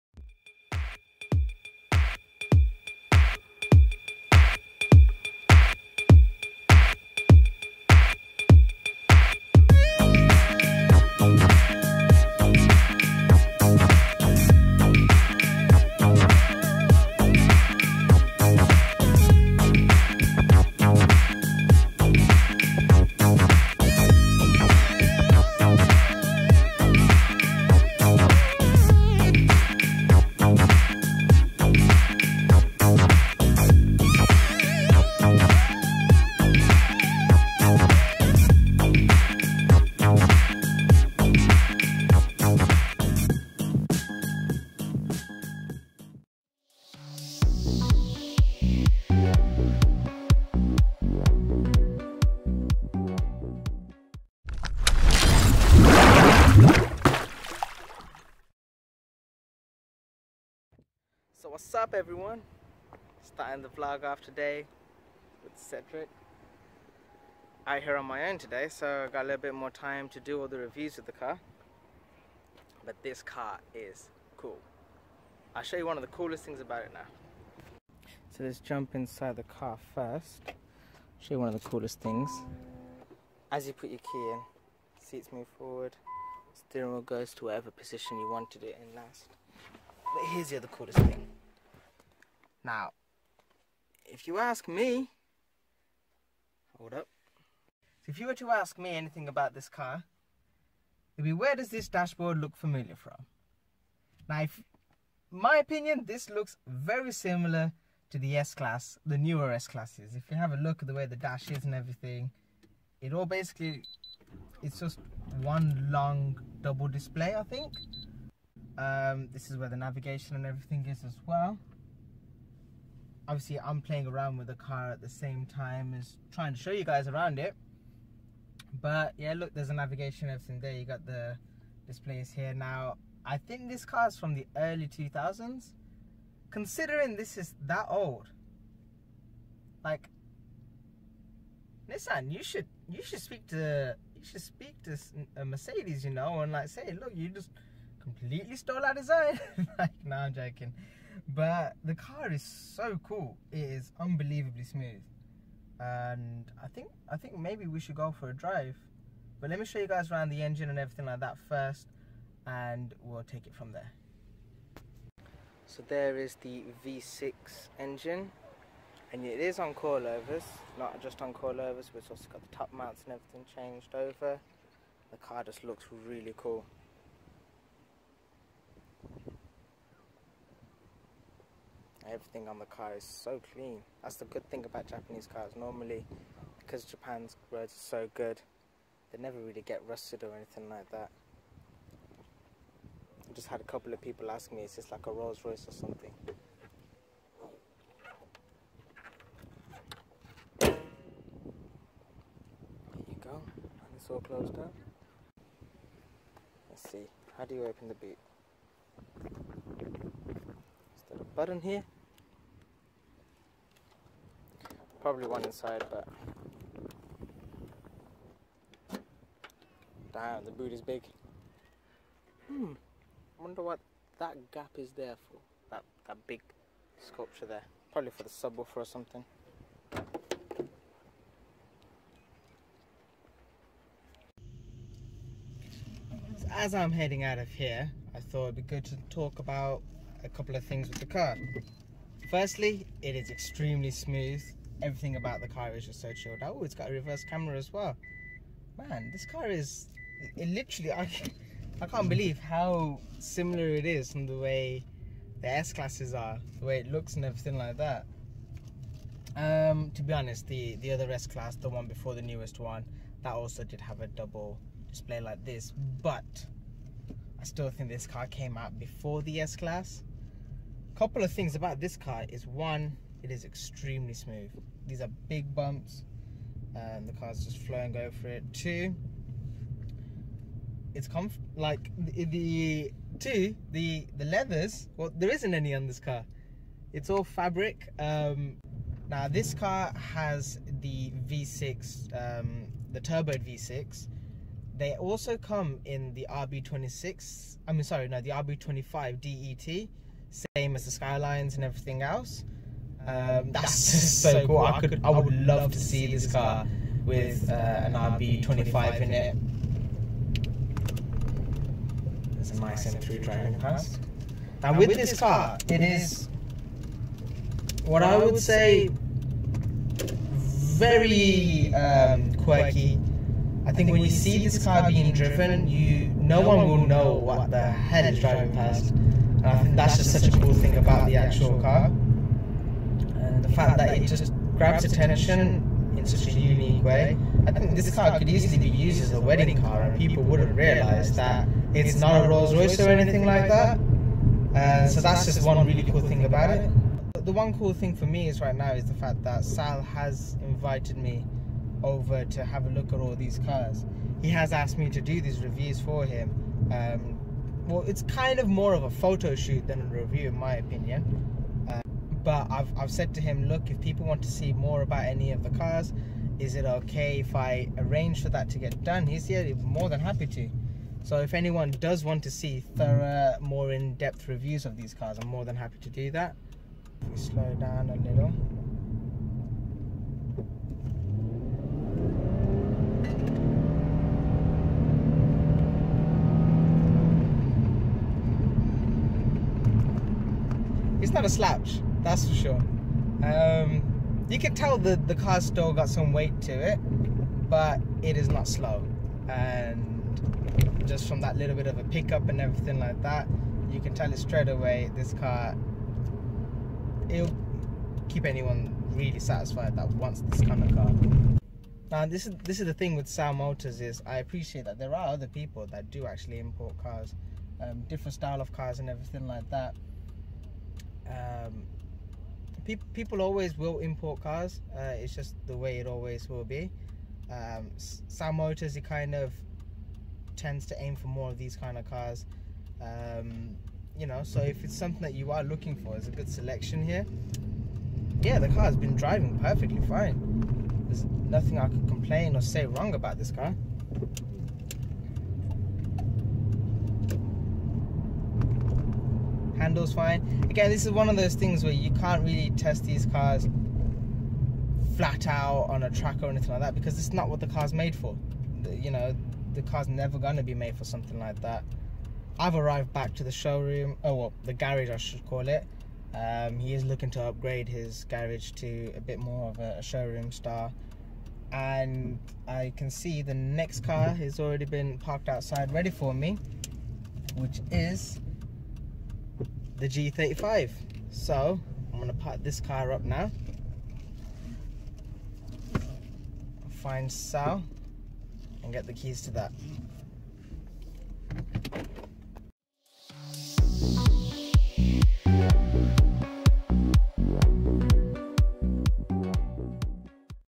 Nissan Cedric Quick Drive & Review | Mr GCC Vlogs | Sal Motors
Got my chance to drive around in a very cool and classy Nissan Cedric.